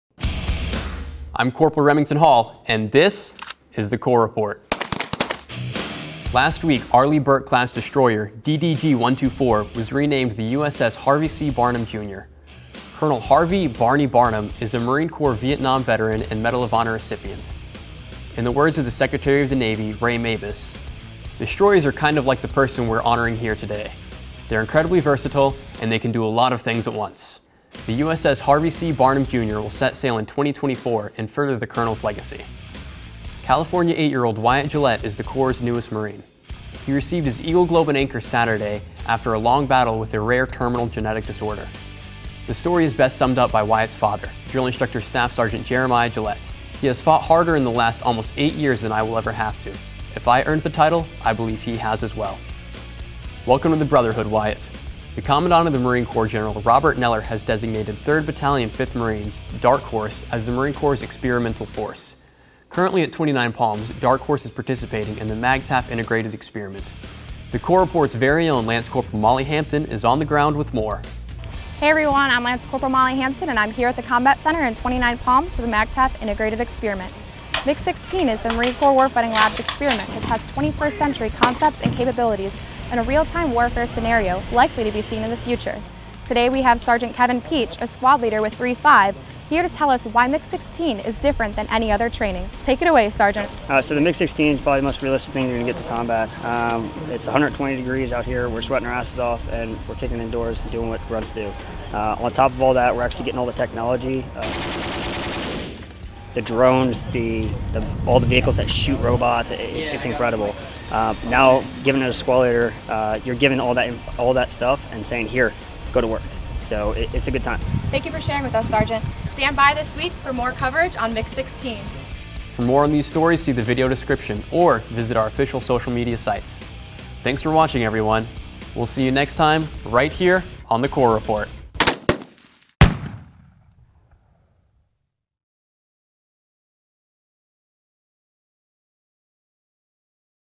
USS Barnum, Honorary Marine, and MIX16 (The Corps Report Ep. 79)
Here's all you need to know about the Corps' latest news.

This week, the the USS Harvey C. Barnum, Jr. (DDG-124) is officially named, Eight year old Wyatt Gillette becomes an Honorary Marine, and Lance Cpl. Molly Hampton sends an update with the Marines of 3/5 and the Marine Corps Warfighting Labs at MAGTF Integrated Experiment 2016.

Filming was done prior the the F/A-18C Hornet crash involving Maj. Richard Norton